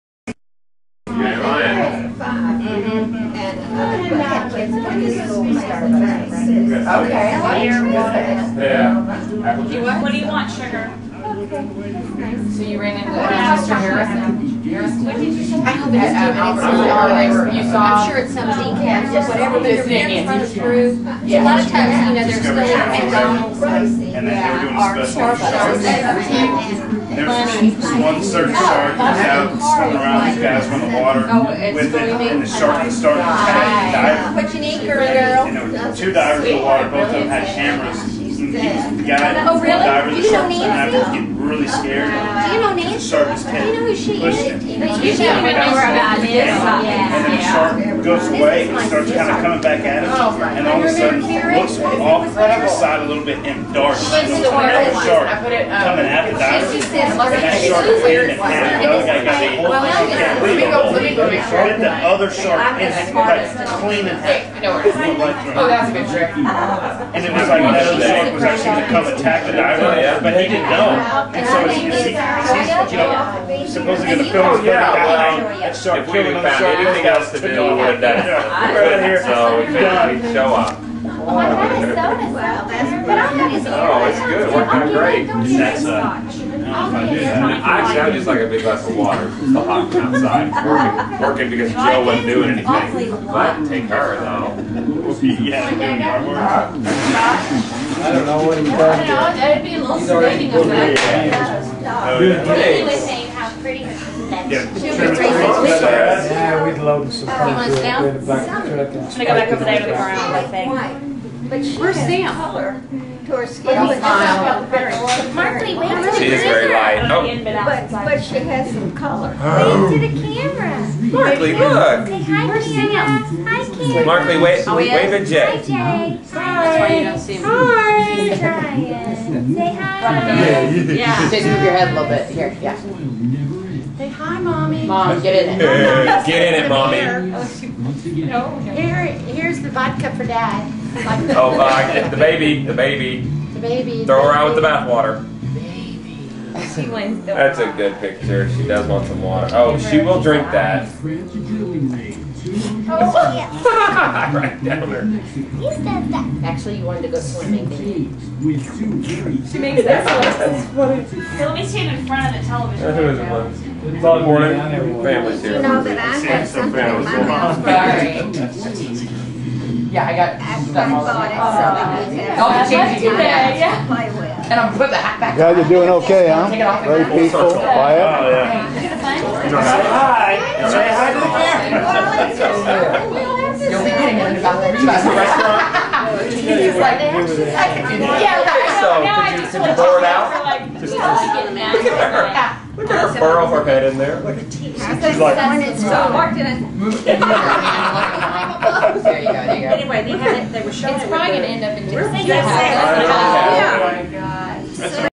Mothers Day Dallas 2007
Peggy, Susan, Joe,  Jason, Denise, Markely, Martha, Chase, Christen, Josephine,  and Nancy, Joe.
May 2007 at Susan's house in Dallas.